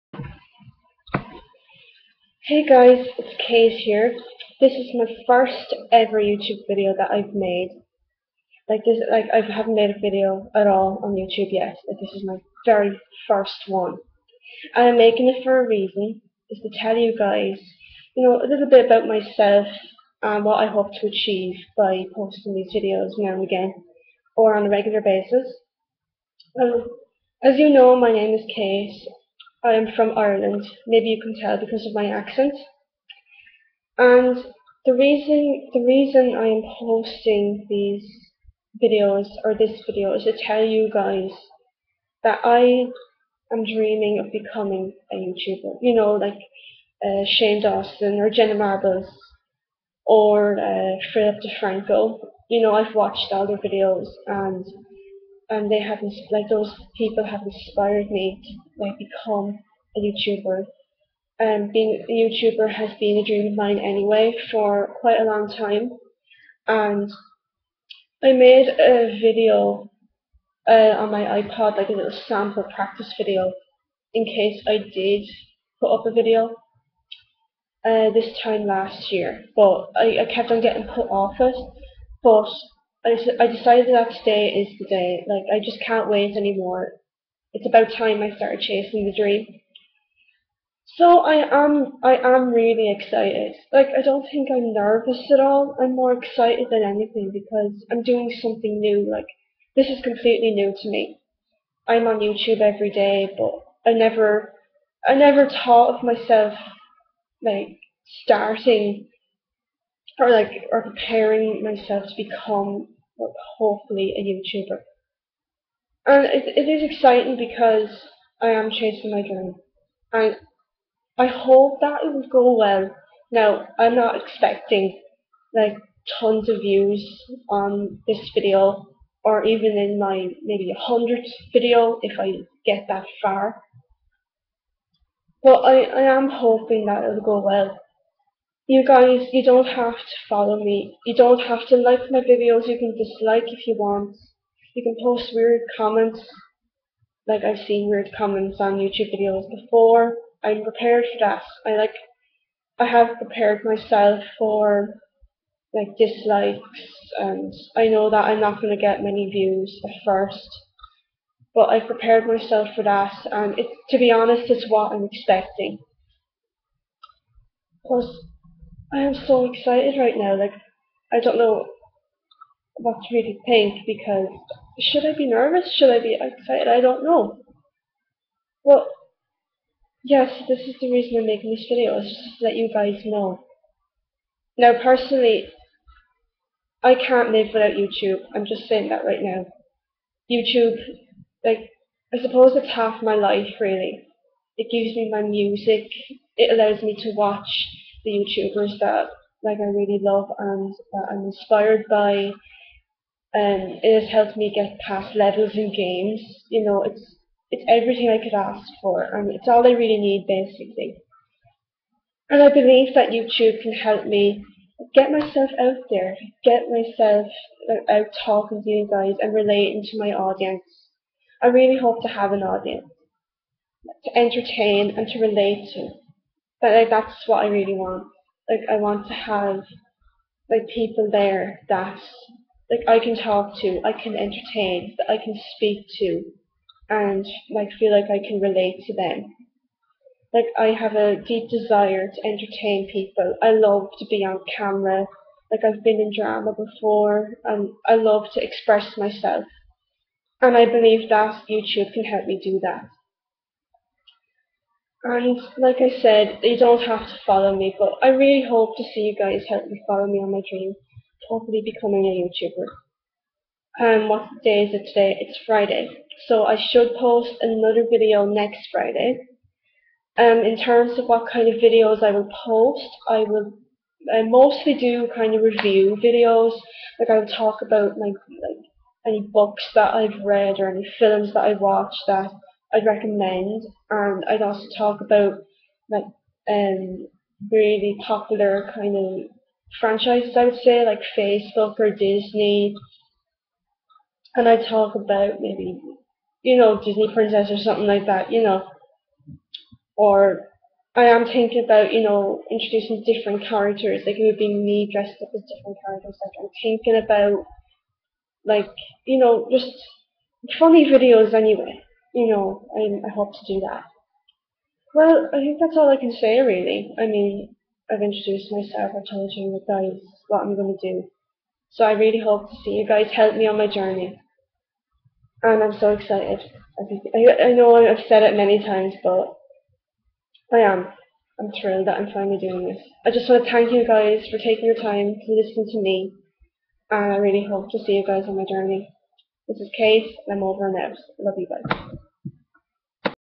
My first ever video!
I have finally gathered enough nerve to post this video! I apologize in advance if you have trouble hearing me my microphone is not exactly the best microphone around but its all i have at the moment!